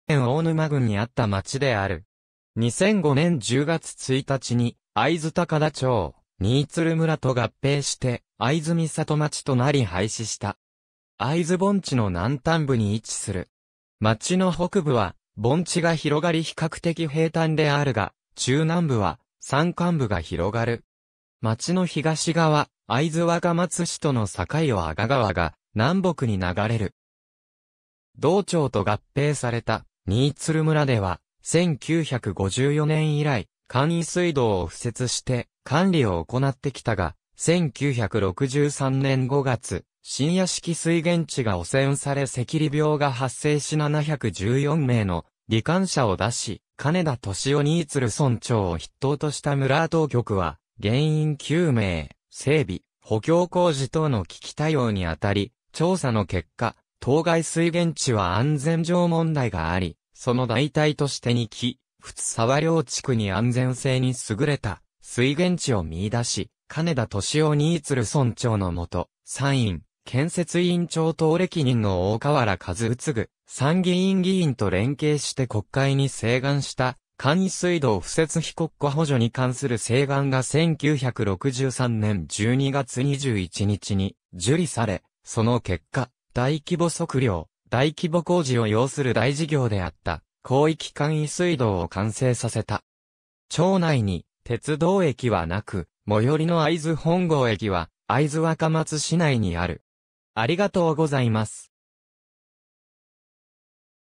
会津本郷町
会津本郷町（あいづほんごうまち）は、福島県大沼郡にあった町である。
2005年10月1日に、会津高田町、新鶴村と合併して会津美里町となり廃止した。
会津盆地の南端部に位置する。
町の北部は盆地が広がり比較的平坦であるが、中南部は山間部が広がる。
町の東側、会津若松市との境を阿賀川が南北に流れる。
同町と合併された新鶴村では、1954年(昭和29年)以来、簡易水道を布設して管理を行ってきたが、1963年(昭和38年)5月新屋敷水源地が汚染され赤痢病が発生し714名の罹患者を出し、金田利雄新鶴村長を筆頭とした村当局は原因究明、整備、補強工事等の危機対応にあたり、調査の結果、当該水源地は安全上問題があり、その代替として二岐、仏沢両地区に安全性に優れた水源地を見出し、金田利雄新鶴村長のもと、参院建設委員長等歴任の大河原一次参議院議員と連携して国会に請願した「簡易水道布設費国庫補助に関する請願」が1963年(昭和38年)12月21日に受理され、その結果、大規模測量、大規模工事を要する大事業であった広域簡易水道を完成させた。
町内に鉄道駅はなく、最寄りの会津本郷駅は会津若松市内（旧・北会津村）にある。